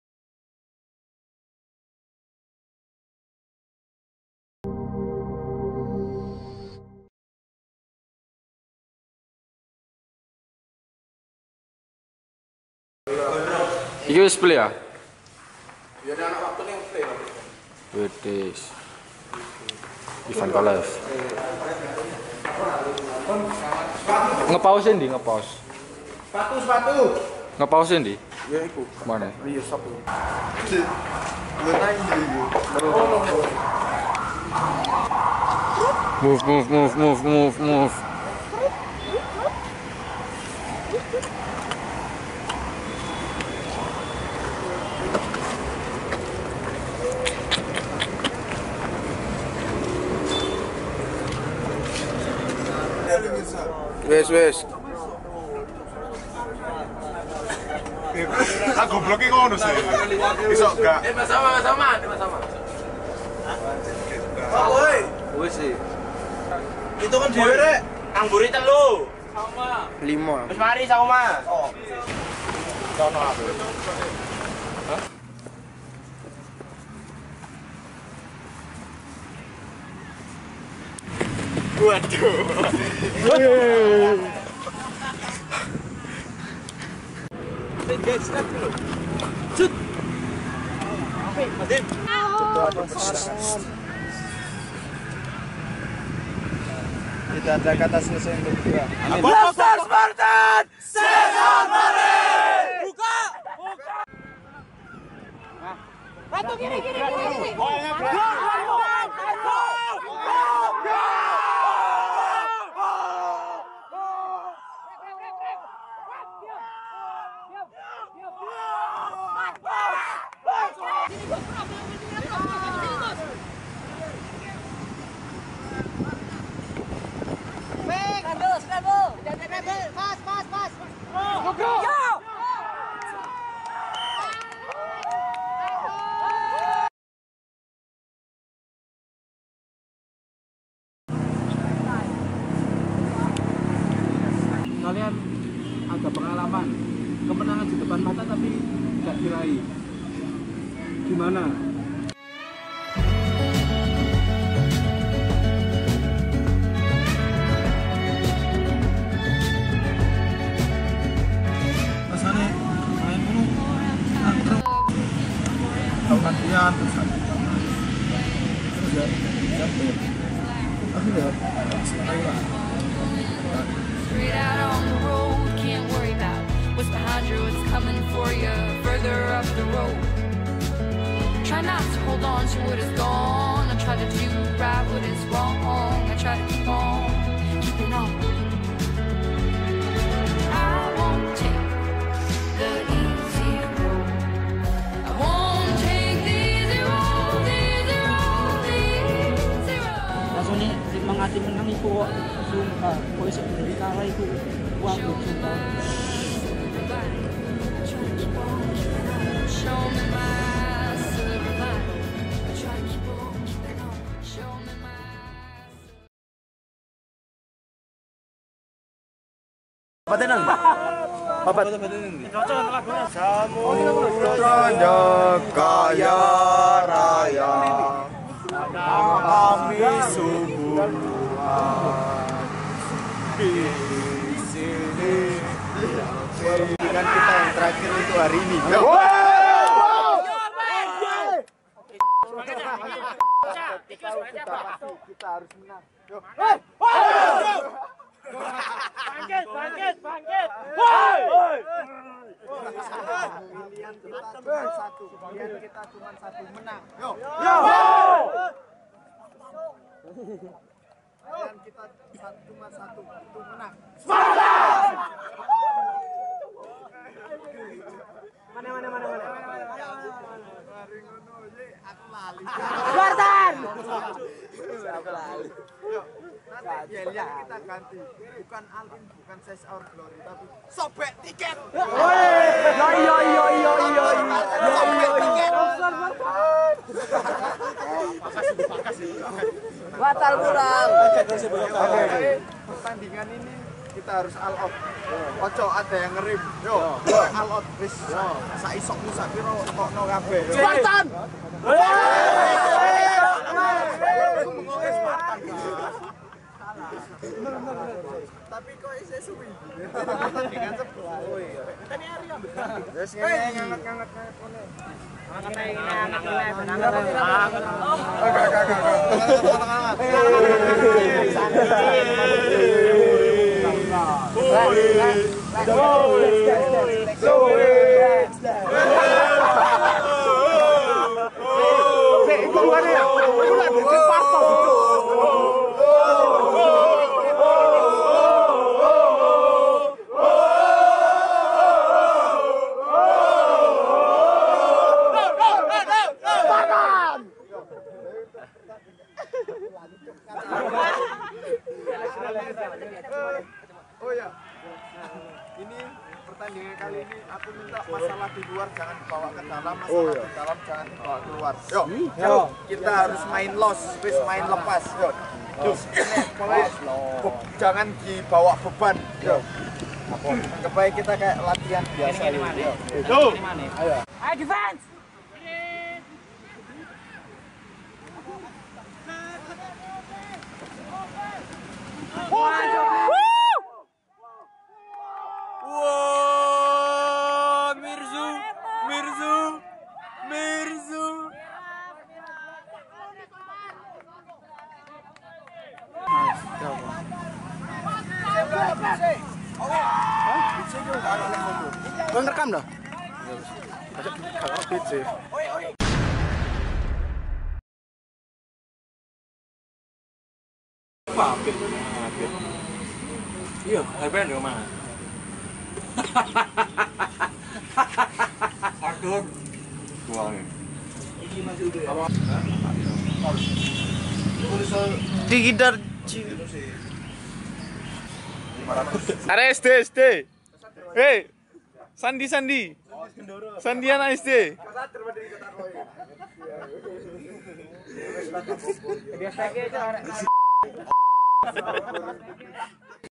Behind the NFG 4 - LS in Bogor ep 2
The fourth National Tournament for LS, NFG 4 Bogor!
NFG 4 Bogor behind the scene!
Keep AllIn and SeizeOurGlory